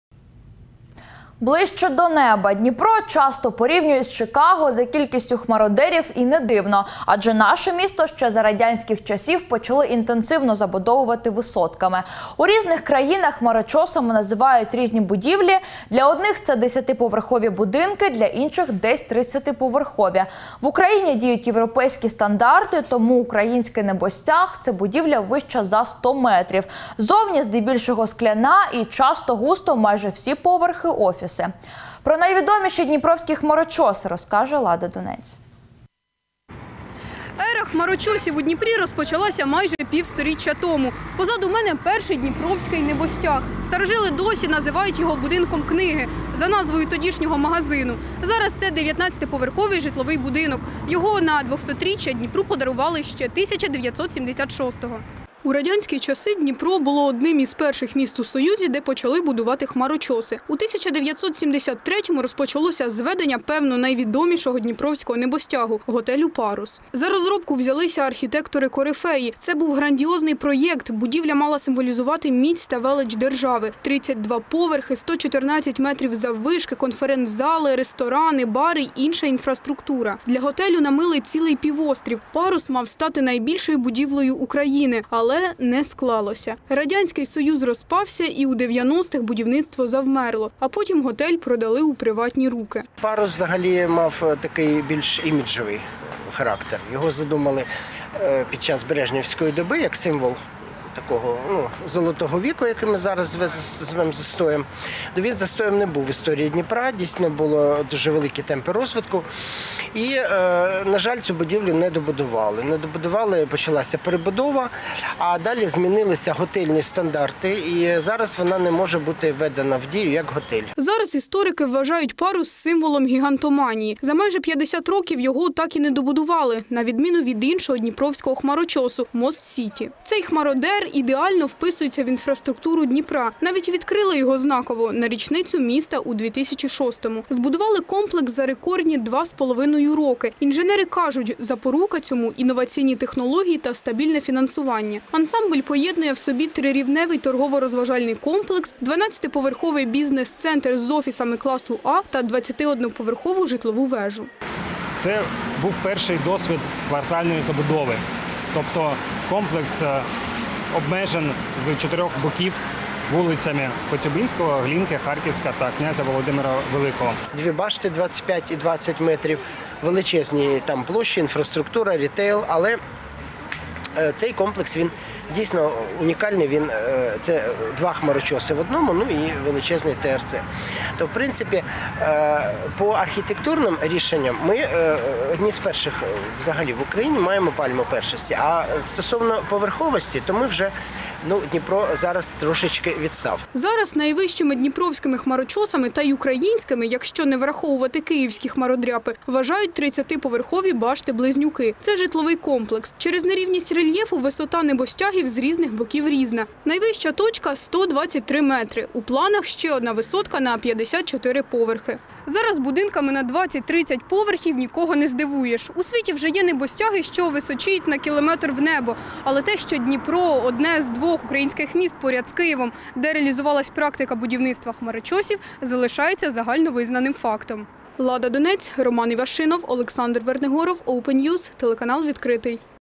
Дніпро - місто хмарочосів
Ближче до неба. Дніпро часто порівнюють з Чикаго за кількістю хмародерів і не дивно, адже наше місто ще за радянських часів почали інтенсивно забудовувати висотками. У різних країнах хмарочосами називають різні будівлі. Для одних - це  десятиповерхові будинки, для інших - десь тридцятиповерхові. В Україні діють європейські стандарти, тому український небостяг - це будівля, вища за сто метрів, зовні - здебільшого скляна і часто-густо майже всі поверхи - офіси.